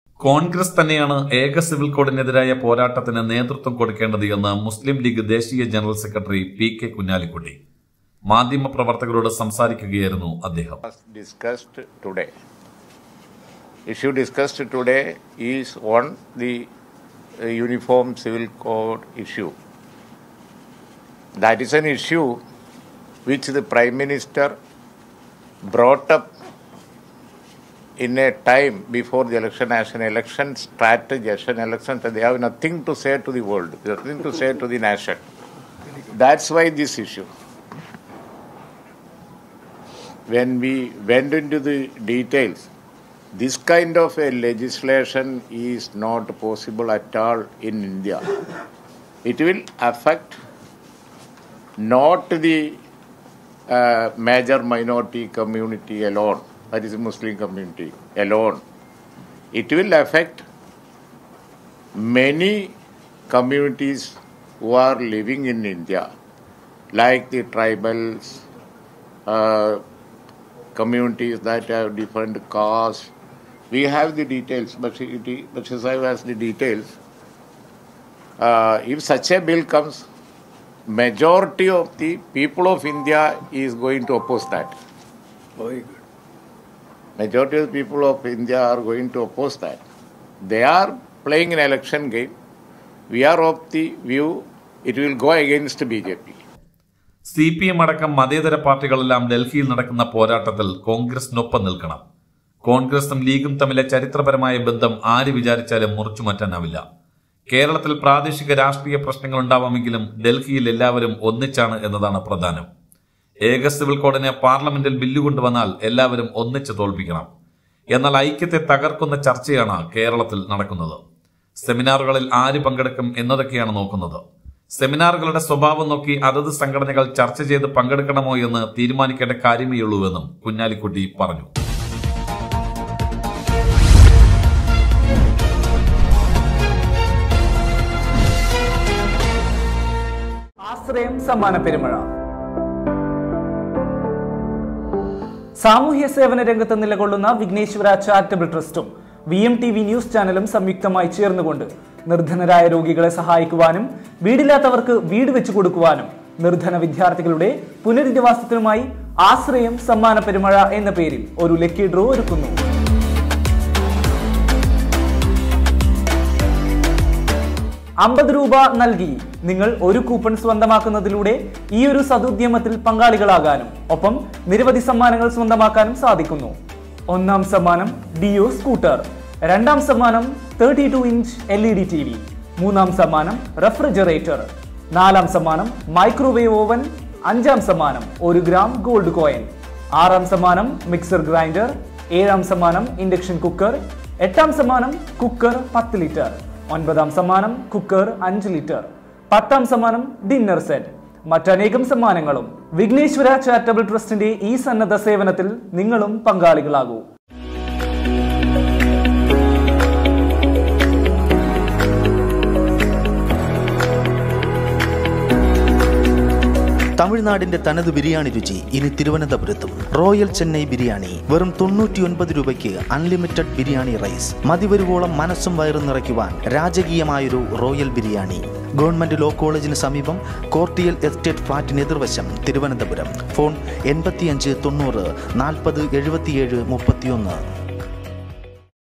ഏക സിവില്‍ കോഡ് ഇന്ത്യയില്‍ അപ്രായോഗികം, നടപ്പാക്കിയാല്‍ പ്രത്യാഘാതമുണ്ടാകും-ലീഗ്......
lPL7UB7XvP5xBXqYlN3yQhI0bAVGvQeYZTG

lPL7UB7XvP5xBXqYlN3yQhI0bAVGvQeYZTG